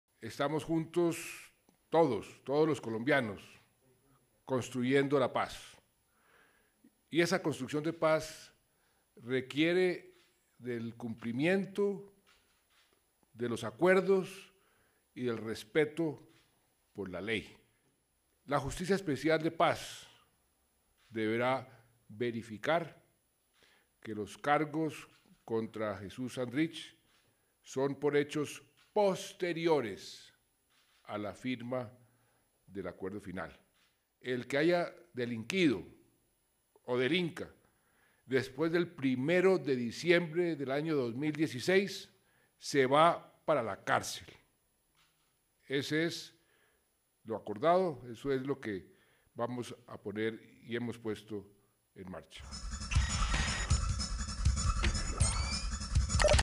‘El que delinca después de la firma del acuerdo irá a la cárcel, es lo acordado’: Santos
Tras la reunión que se llevó a cabo hoy con los principales líderes del partido político FARC, el presidente Juan Manuel Santos se refirió al tema de la captura de Jesús Santrich por parte de la Fiscalía en días pasados.